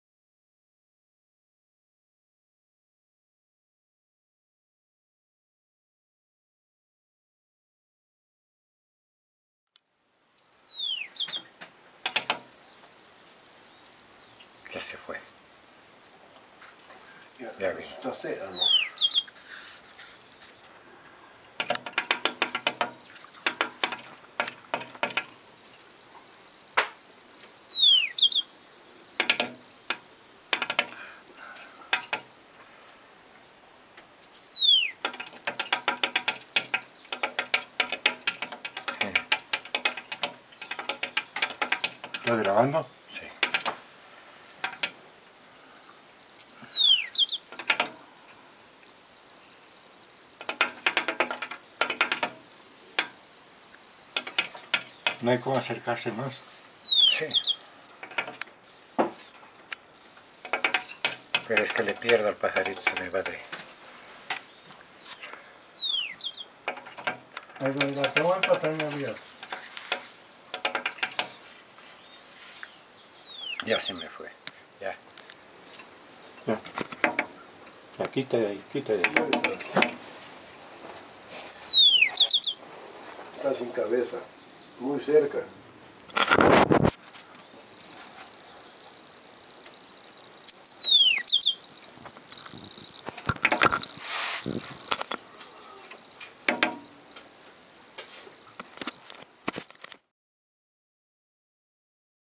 Gorrion despistado en Yaruqui
Una mañana llegando a mi casa de campo en Yaruqui, entrando a la sala, escucho unos golpes en el vidro, me asomo aver y tenía a un pequeño gorrón despistado golpeando el vidrio reflectivo, aquí unos instantes de video que recuerdan ese día.